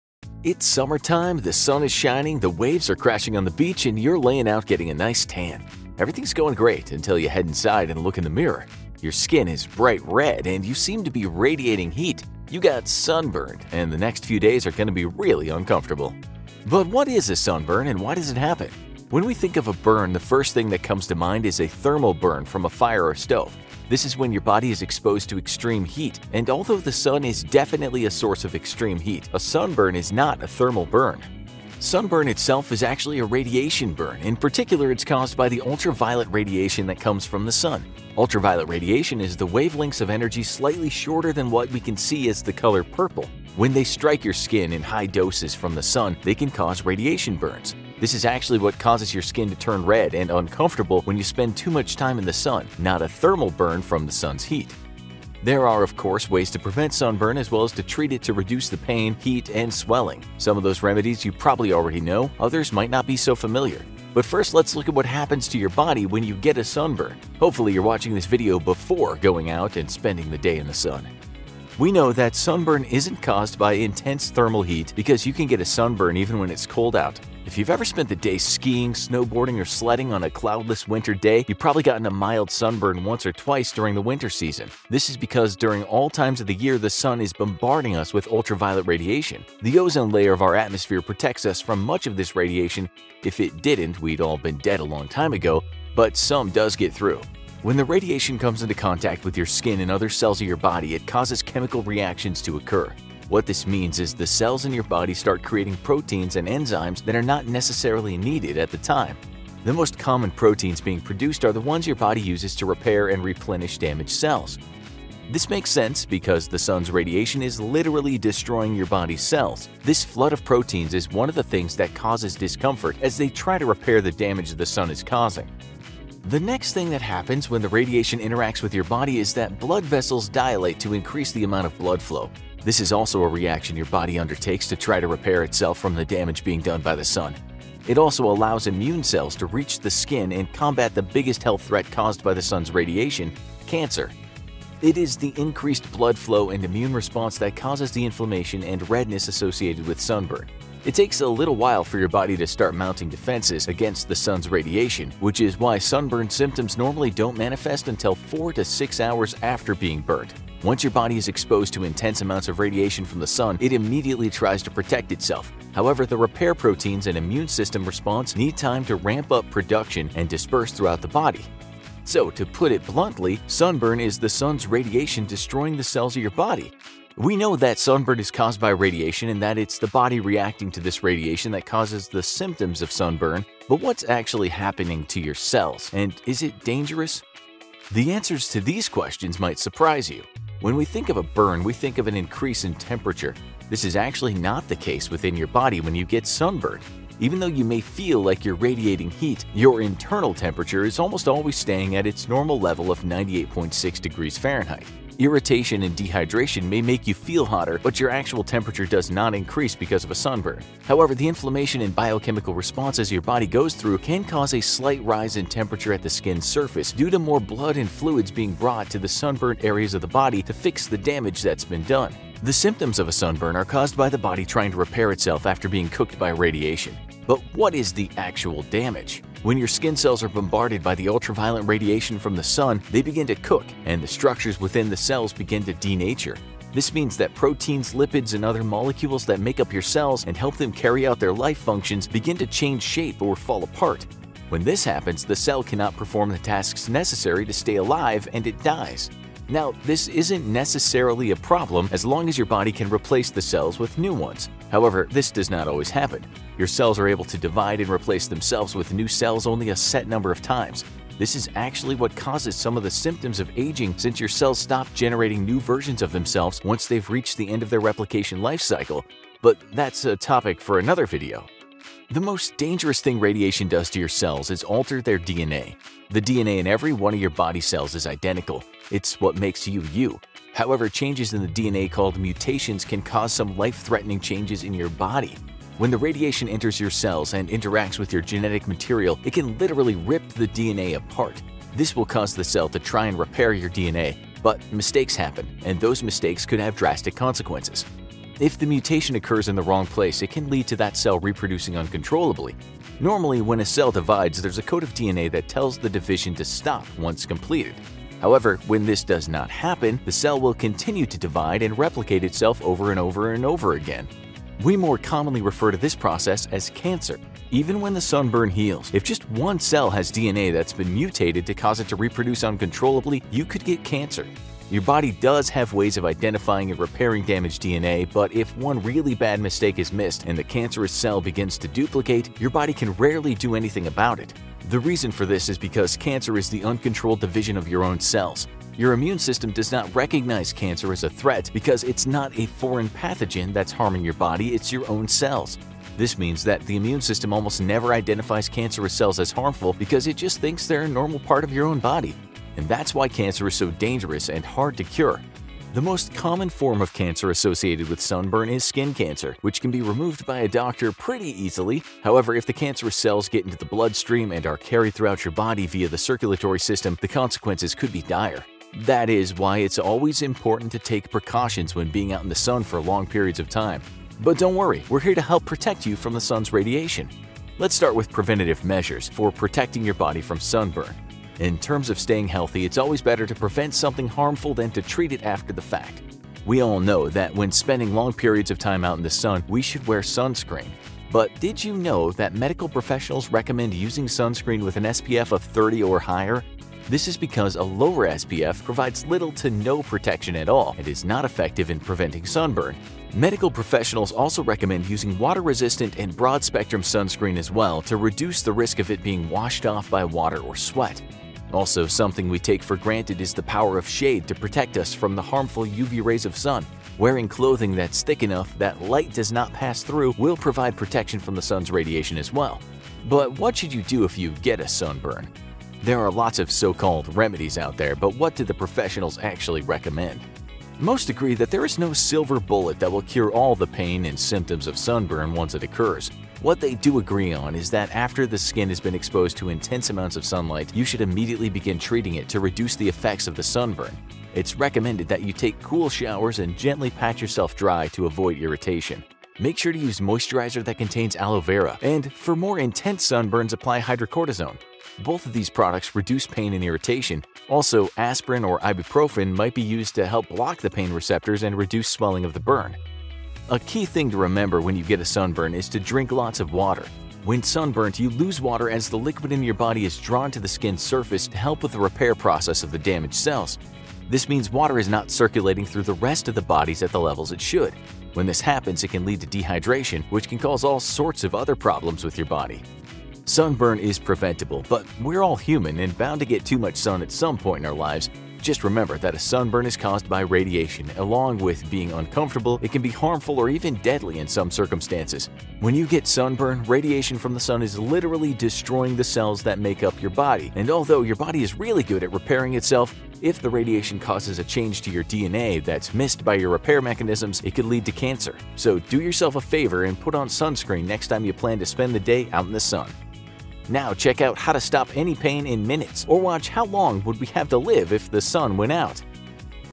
What Actually Happens When You Get a Sunburn
It's summertime, and after a year of being locked up in quarantine it's time to step outside (safely) and get some ultraviolet rays of sunshine. But before you go soaking up that sun you'd better get that sunscreen on, because otherwise your skin will literally burn, and the damage that can be caused by a sunburn can actually be serious. Find out what sunburns really are doing to your skin right now!